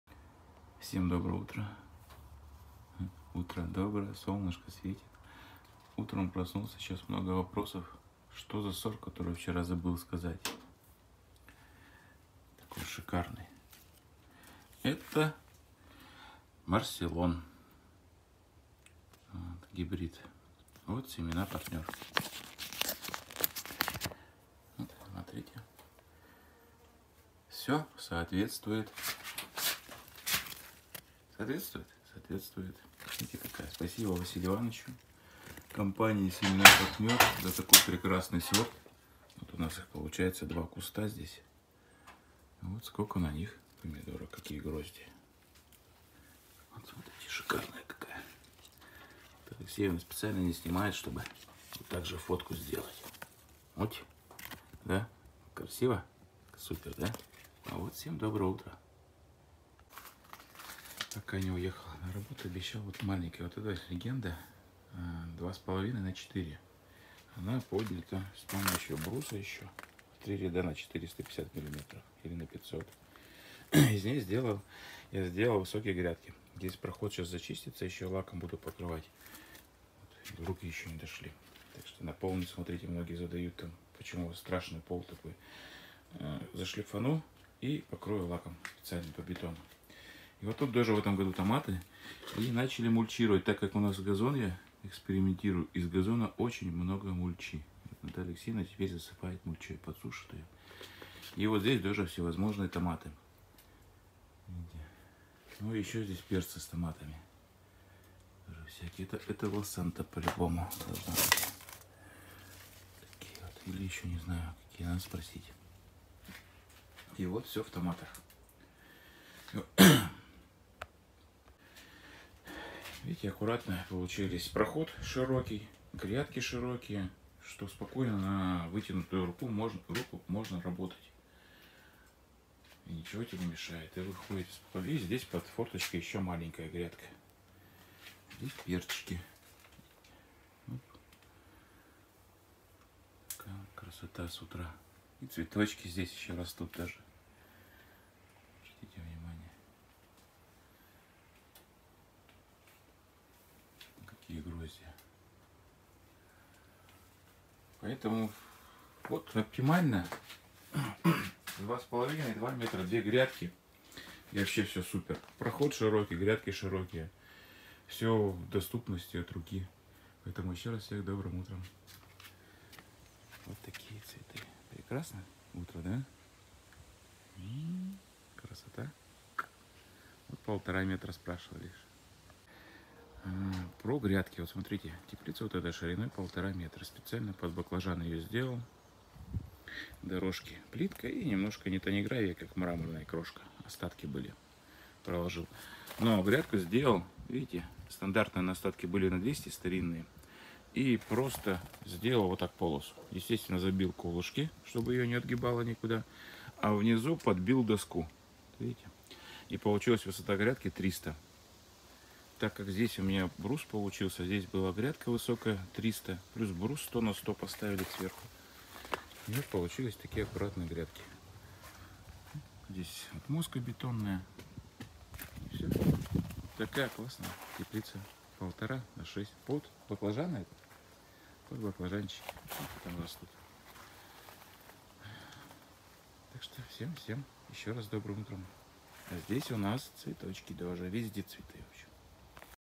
ФАЗЕНДА. ТОМАТНОЕ ЦАРСТВО ЧАСТЬ 2.
Наша фазенда, рассказываем про томаты